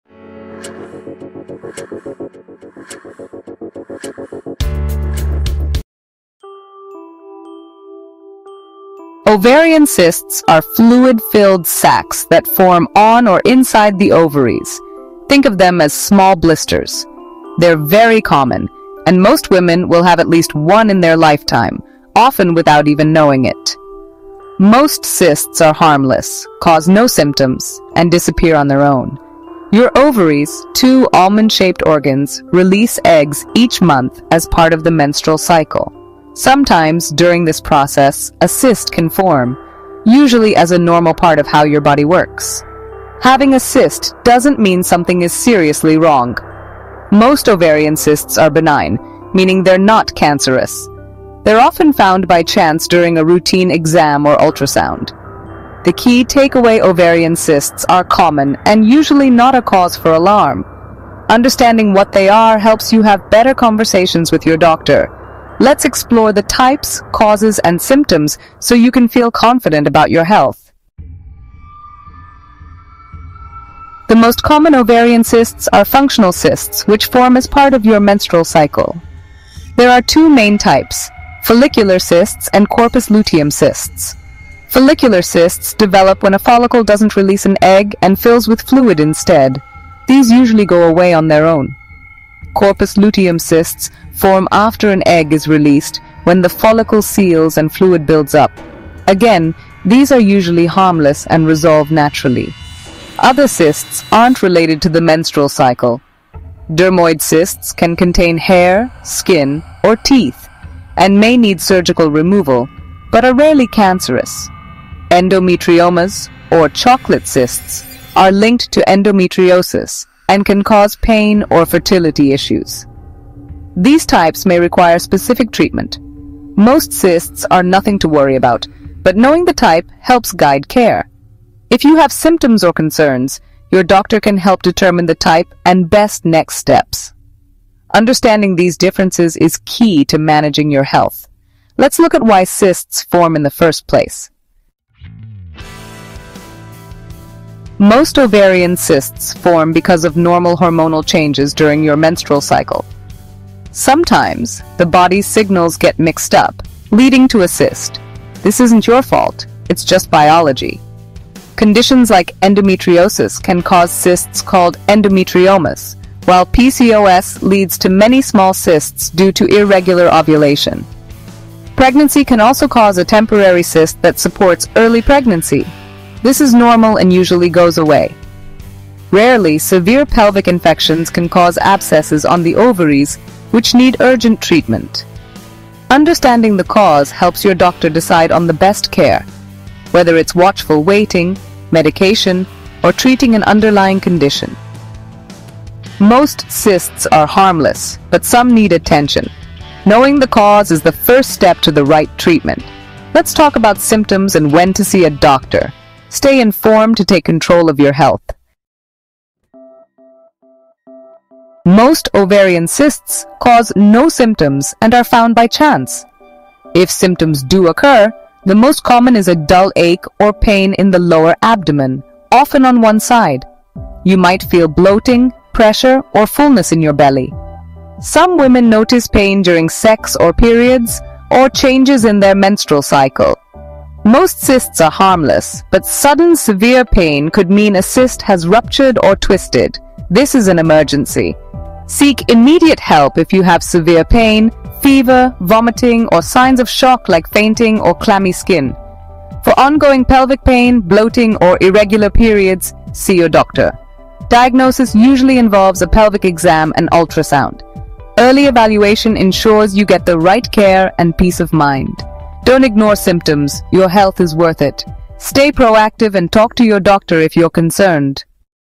"Cysts in Ovaries: Silent Danger Every Woman Should Know! ⚠️ PCOS PelvicPain OvarianHealth"
🔍 Are you or someone you love struggling with unexplained abdominal pain or irregular periods?
You might be ignoring a serious condition — Ovarian Cysts! These fluid-filled sacs can be harmless, but sometimes they can be a sign of PCOS, endometriosis, or even ovarian torsion.

👩‍⚕️ In this video, we break down:
✅ What are ovarian cysts?
✅ Causes and symptoms ⚠️
✅ When to worry and seek medical help
✅ Natural tips and medical treatments

🎯 Must-watch for all women, mothers, and daughters!
Stay aware. Stay healthy. 💖